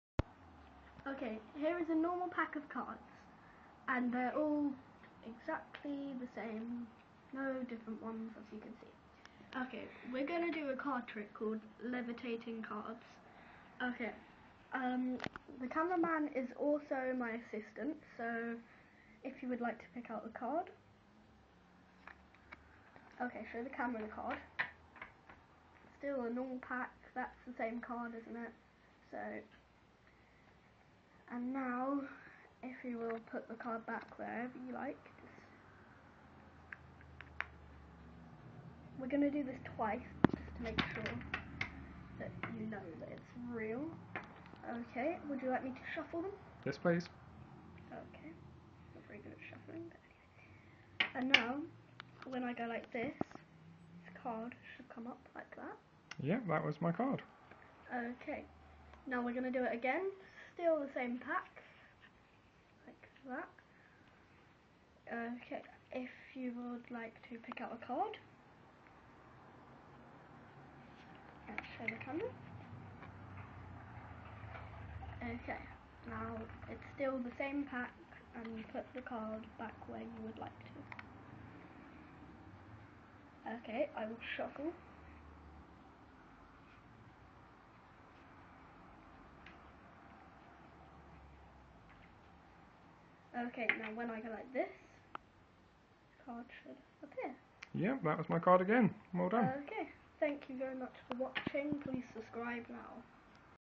Levitating card trick!
This is a simple but fascinating trick that I learnt from a website,But how do I do it comment if you know and please suscribe,Btw I had just washed my hair thats why its wet.